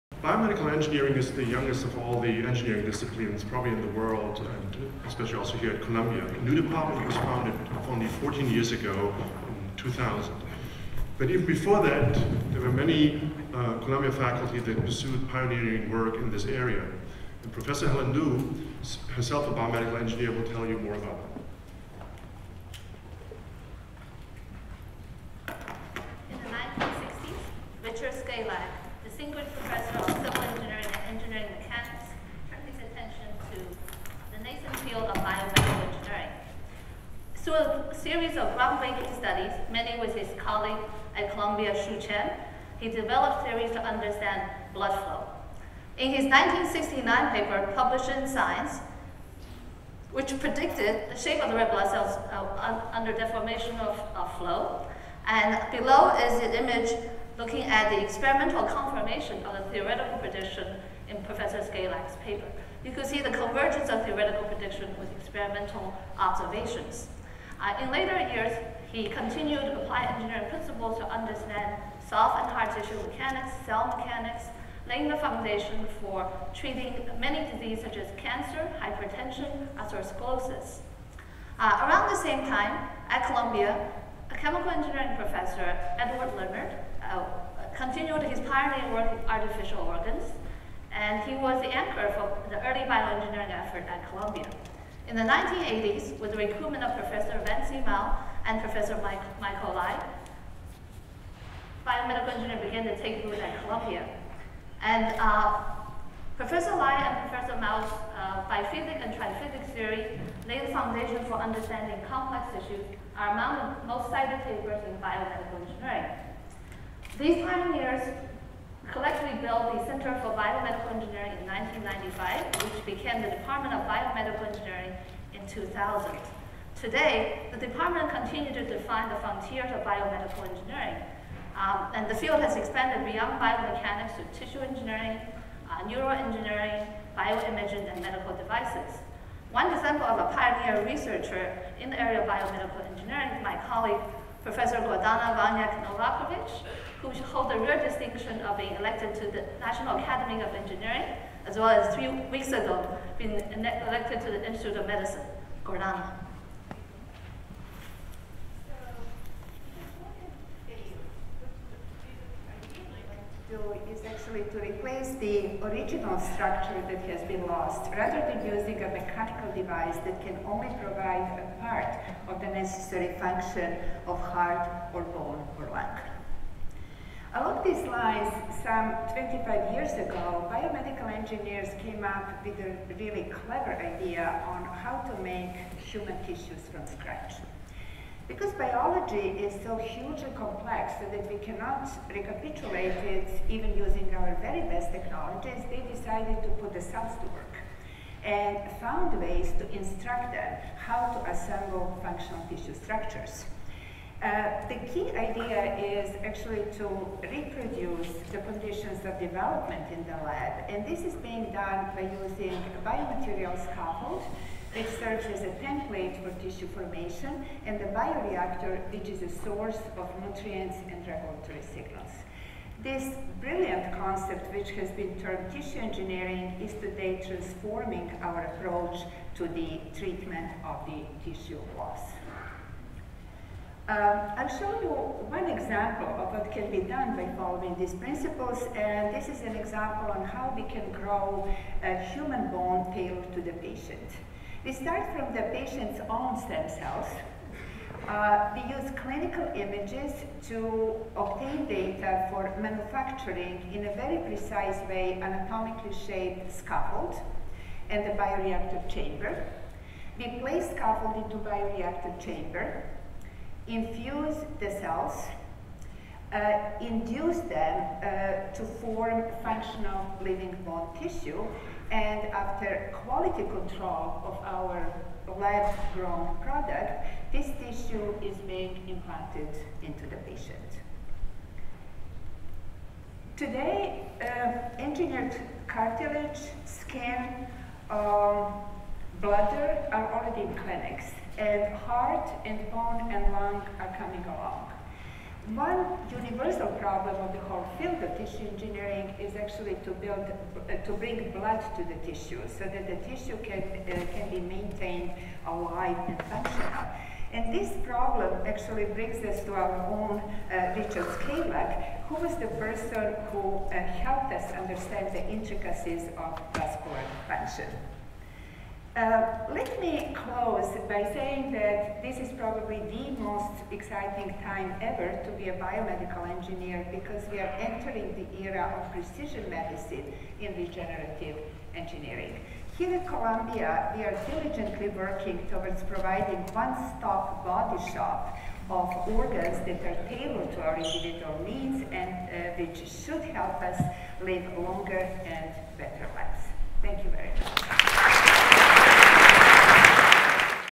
150th Anniversary Symposium: Columbia's Engineering Renaissance. Biomedicine. 07
Pioneers of Biomedical Engineering / Precision Medicine: Regenerative Engineering
Helen H. Lu, Professor of Biomedical Engineering
Gordana Vunjak-Novakovic, The Mikati Foundation Professor of Biomedical Engineering and Professor of Medical Sciences in Medicine